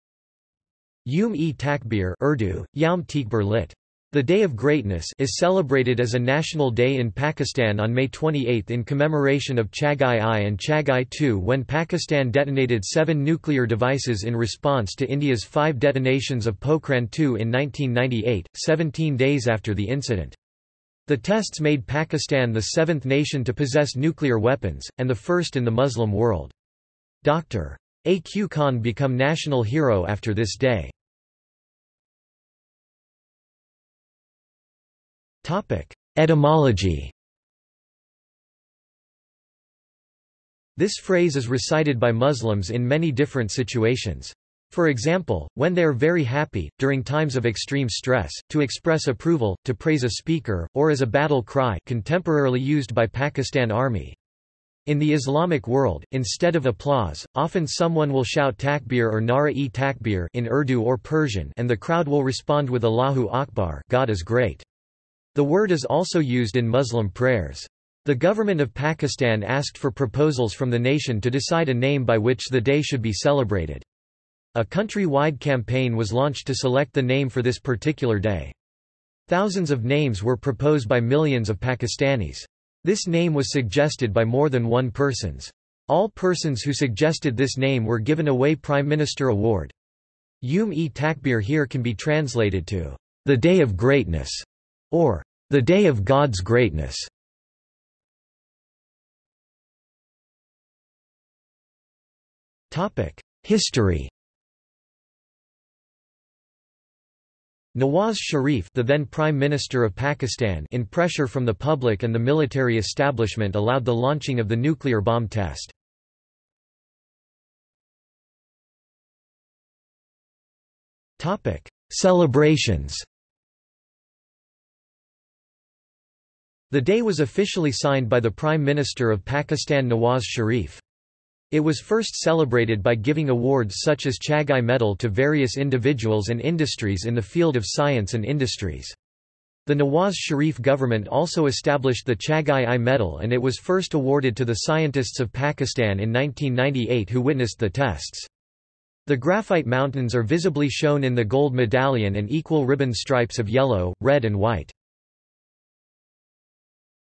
Youm-e-Takbir | Wikipedia audio article
This is an audio version of the Wikipedia Article:
Youm-e-Takbir

00:00:31 1 Etymology
00:01:39 2 History
00:01:57 3 Celebrations

- Socrates

SUMMARY
Youm-e-Takbir (Urdu: یوم تکبیر‬‎; lit. The day of greatness) is celebrated as a national day in Pakistan on May 28 in commemoration of Chagai-I and Chagai-II when Pakistan detonated seven nuclear devices in response to India's five detonations of Pokhran-II in 1998, 17 days after the incident. The tests made Pakistan the seventh nation to possess nuclear weapons, and the first in the Muslim world. Dr.AQ khan become national hero  after this day